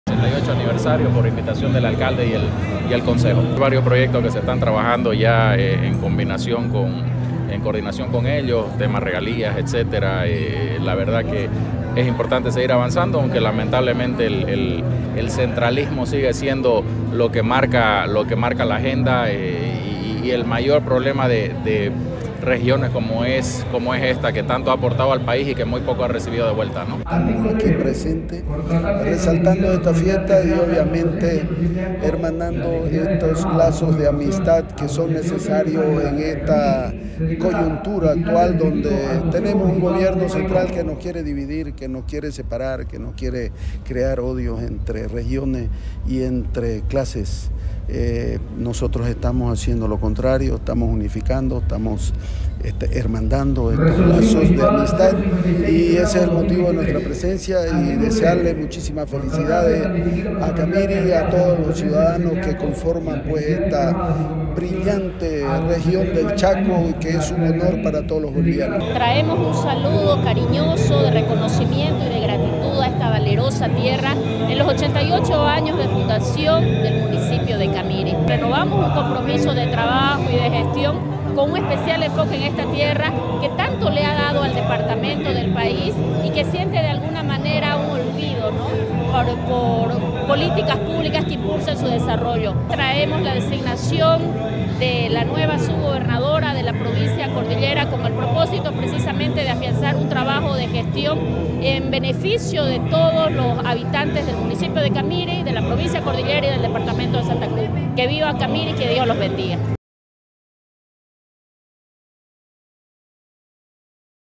Camiri celebra 88 años de fundación y designan a nueva Subgobernadora
_Reclamo."El centralismo se llevó toda la plata, las regiones que tanto aportaron al país siguen siendo pobres".__

 *Camiri, 11 de julio de 2023.-* La 'capital petrolera' festeja sus 88 años de fundación. Fue creada el 12 de julio de 1935 y se encuentra a 290 km al sur de Santa Cruz de la Sierra. Hoy Camiri recibe a las autoridades departamentales que junto a las autoridades locales se unen a la población camireña para celebrar en grande con un programa de actividades preparado por el gobierno municipal, a la cabeza de su alcalde, David Anzaldo.

Los asambleístas departamentales de Santa Cruz, Zvonko Matkovic, presidente de la ALD, Oscar Nelson Feeney, Vicepresidente y Paola Aguirre, asambleísta secretaria participaron de la Sesión de Honor y actos protocolares de rigor donde recibieron reconocimientos por parte de las autoridades municipales camireñas.

"El centralismo se llevó toda la plata, las regiones que tanto aportaron al país siguen siendo pobres, y eso es porque no pueden manejar el destino de sus recursos", manifestó Matkovic a tiempo de saludar y felicitar a los camireños.

El máximo representante del Órgano Legislativo cruceño, tambien dijo que a través de la Gobernación y la Asamblea Departamental se coordinan varios proyectos que se están trabajando con el tema de las regalías y es importante seguir avanzando, aunque lamentablemente el centralismo sigue siendo el que marca la agenda, aseguró.

Por su parte el vicepresidente de la ALD, Oscar Feeney, en la ocasión manifestó que es importante reforzar los lazos de amistad. "En los 88 años de Camiri estamos aquí presentes, para extender lazos de amistad y hermandad frente a un Gobierno Central que nos quiere dividir entre regiones y entre clases, nosotros estamos haciendo todo lo contrario", manifestó Feeney.

La asambleísta Paola Aguirre también intervino en las actividades. "Venimos en representación del Gobierno Autónomo Departamental de Santa Cruz y de la Asamblea Legislativa Departamental, traemos un saludo cariñoso y un reconocimiento y gratitud a esta valerosa tierra en los 88 años de fundación del municipio de Camiri, también en representación del gobernador Luis Fernando Camacho que nos pide trabajar por el departamento y de esta manera ratificamos nuestro compromiso", manifestó Aguirre.

 Asimismo, destacó la designación de la nueva Subgobernadora de la provincia Cordillera, Teresa Galarza Sánchez, con el propósito de afianzar un trabajo de gestión en beneficio de todos los habitantes del municipio camireño. En Santa Cruz necesitamos trabajo de cooperación entre los diferentes niveles del estado y particularmente entre la Gobernación de Santa Cruz y la Subgobernación. En ese sentido nosotros ratificamos nuestro compromiso de seguir trabajando para atender las necesidades prioritarias de la población y una vez más felicitamos al valeroso pueblo de Camiri en sus 88 años de Fundación", finalizó la autoridad.

También se realizaron como parte del programa, actividades artísticas, deportivas, ferias donde se muestra la riqueza cultural, productiva e histórica, de la región chaqueña.